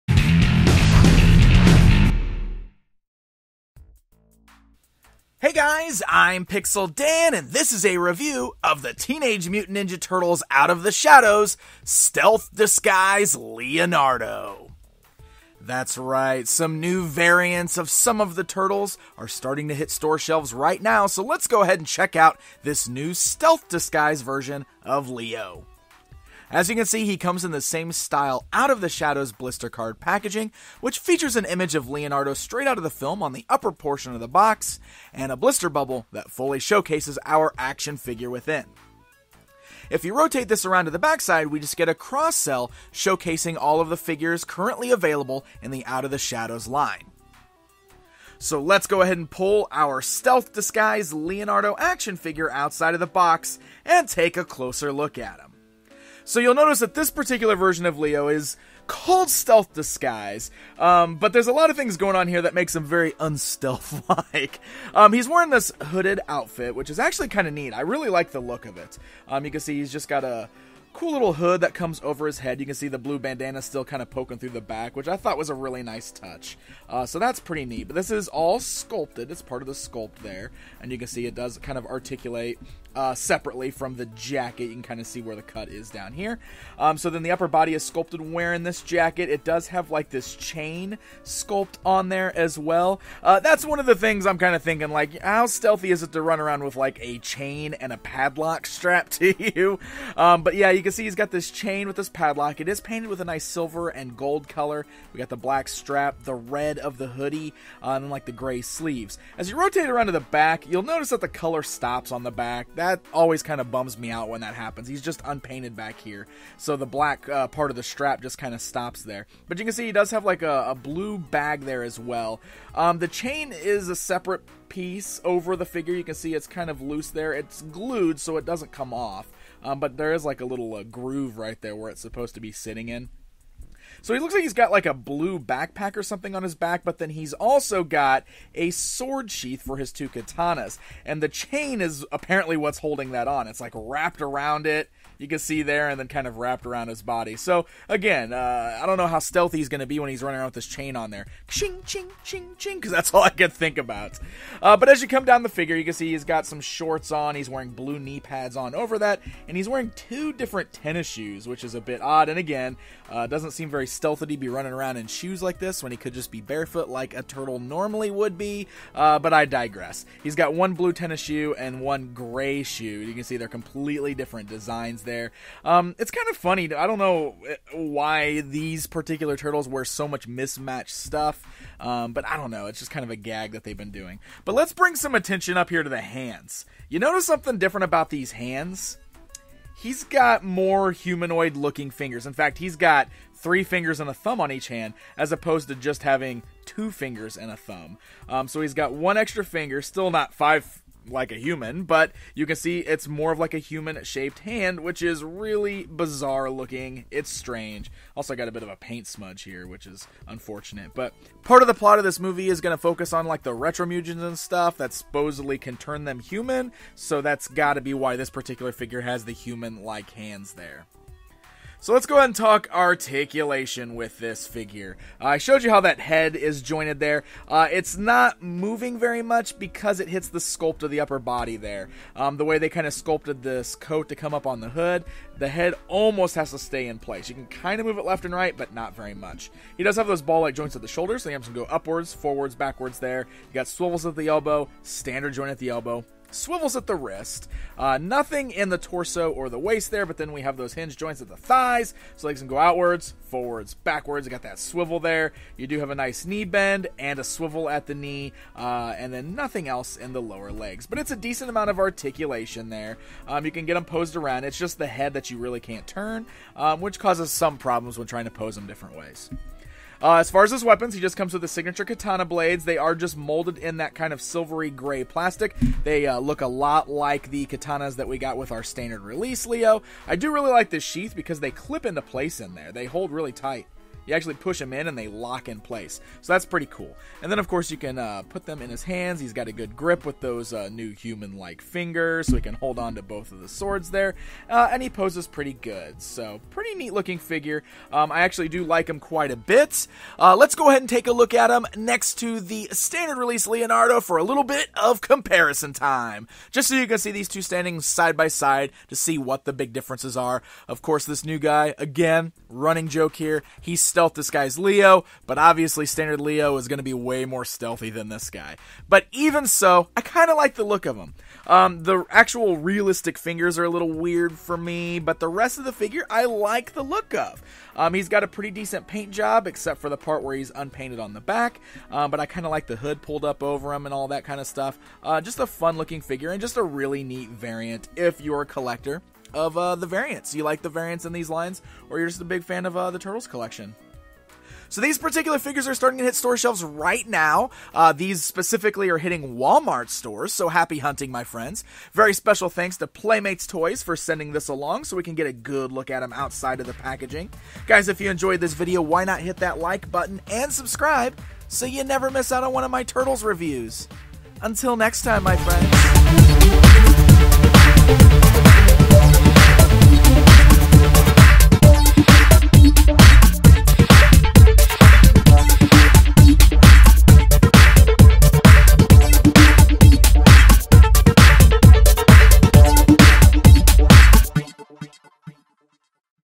Stealth Disguise Leonardo Teenage Mutant Ninja Turtles Out of the Shadows Figure Review
Join Pixel Dan for a look at the new Stealth Disguise Leonardo figure from TMNT Out of the Shadows!